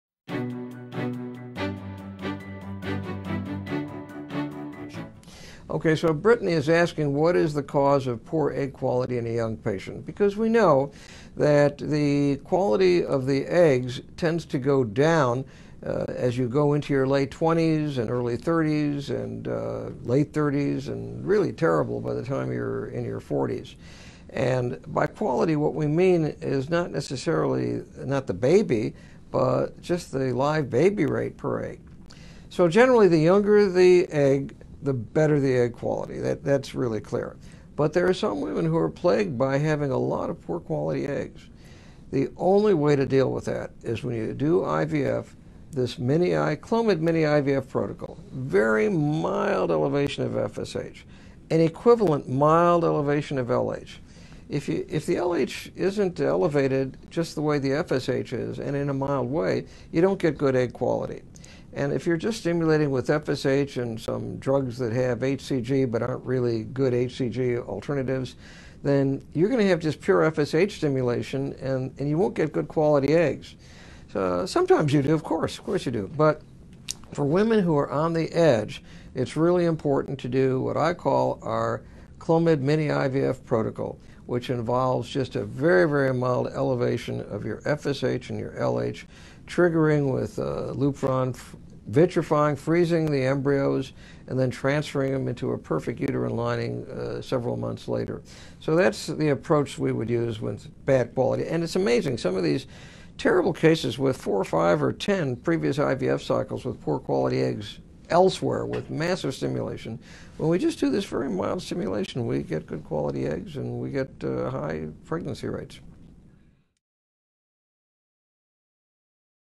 What are causes of poor egg quality in young patients
Dr. Silber answers a viewer question from Facebook Live. The viewer asked about causes of poor egg quality in a young patient.